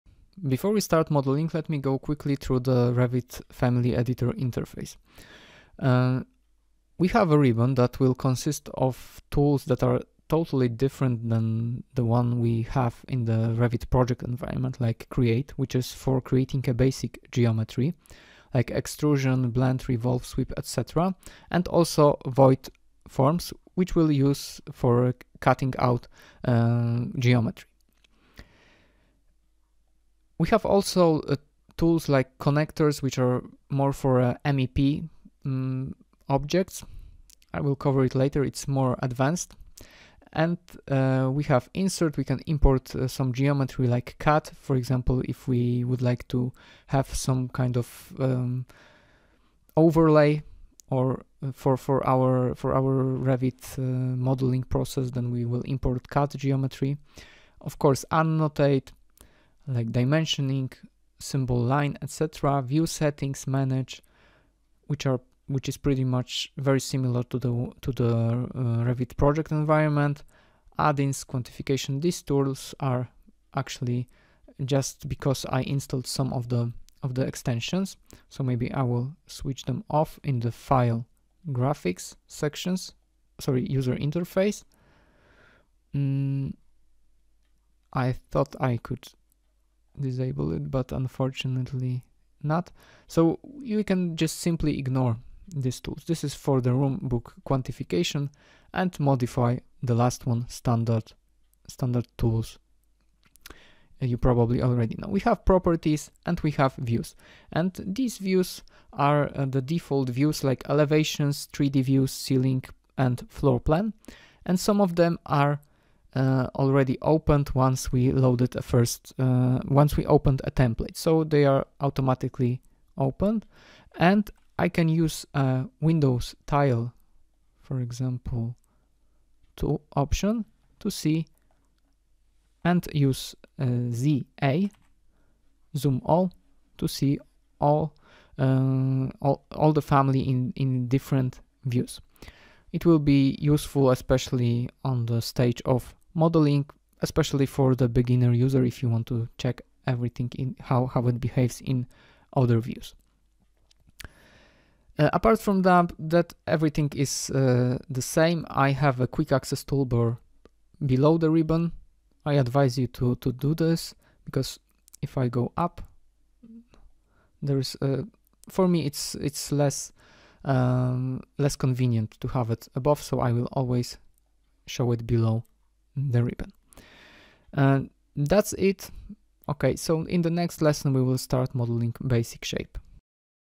REVIT FAMILIES COURSE: Module 1 | 3. Family editor interface
3rd lesson of Revit 2023 family course. I will introduce you to family editor interface which looks a lot different than project envoirenment. You will find there different tools and predefined settings.

📰 Helpful articles with step by step tutorials
⭐Premium content published only on the blog